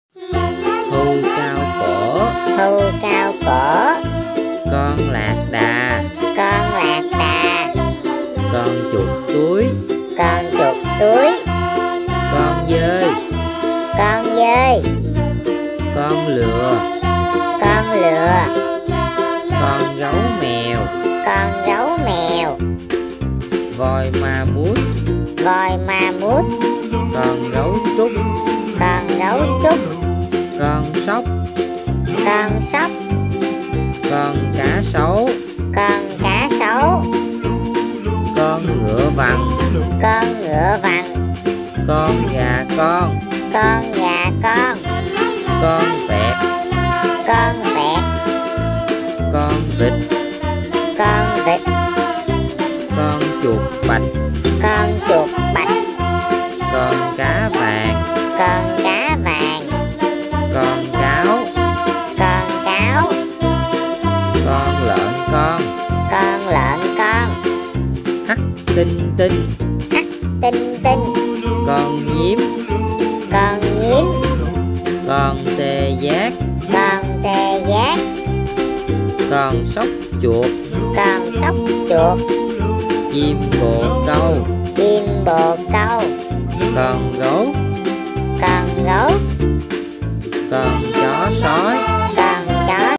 Wild Animals Vocabulary । Từ Vựng Về Động Vật Hoang Dã । Học Đánh Vần Có Phụ Đề Tiếng Anh Cho Bé
VietNam Alphabets to your kids. This video helps your kids to learn VietNam Alphabet in an easy way.
(Bảng chữ cái tiếng Việt cho con của bạn. Video này giúp con bạn học Bảng chữ cái Tiếng Việt một cách dễ dàng.)

Copyright by Top 1 IQ